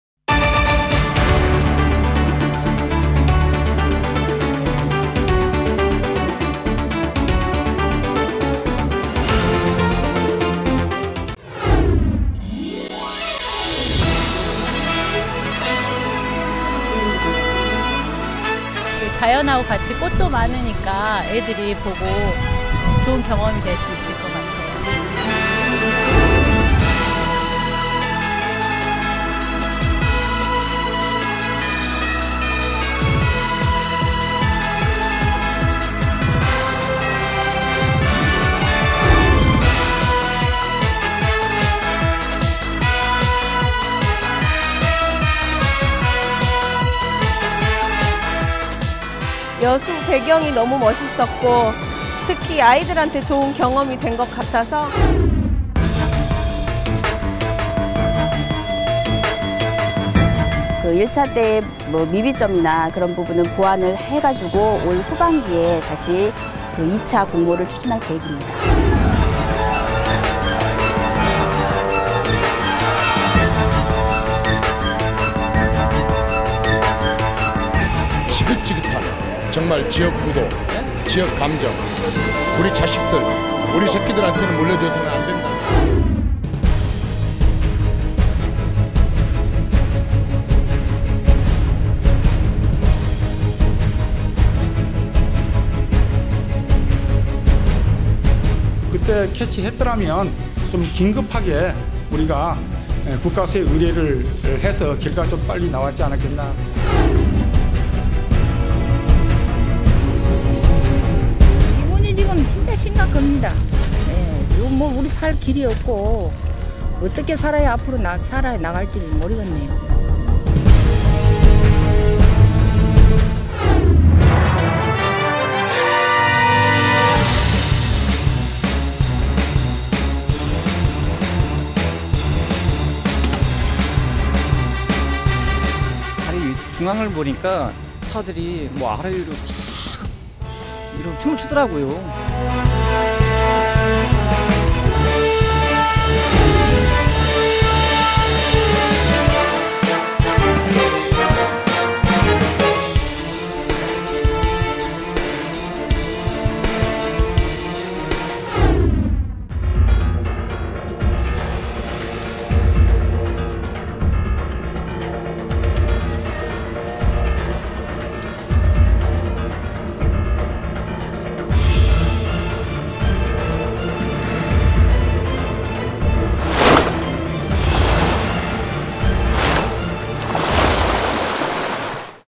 [여수MBC] 2014 영상으로 본 10대뉴스_순천만정원 개장, 이정현 당선, 유병언
아듀 2014, [여수MBC] 영상으로 본 10대뉴스!!
1. 순천만정원 개장 - "정원도시 꿈꾼다"
2. 여수 순천 광양 행정협의회 - 7년 만에 재개
3. 국내 최초 해상케이블카 - 논란 끝 '개장'
4. 여수세계박람회장 - 사후활용 '또 표류'
5. 전남동부지역, 정치판도 변화 -'이정현' 새누리당 후보 당선!!
6. 유병언 변사체 발견 - 검경 부실수사 논란
7. 우이산호 사고 - 기름으로 뒤덮인 여수
8. 이순신대교 '흔들흔들'...불안
9. 광양항 국내 2위 항만 '위협'
10. 철강, 석유화학 등 지역 주력산업 침체

"여수MBC News+ on YouTube - - - COMING SOON !!"

*스마트폰 웹하드 이용 제보: Webhard App설치 후 이용
ID:ysmbc / PW:ysmbc1011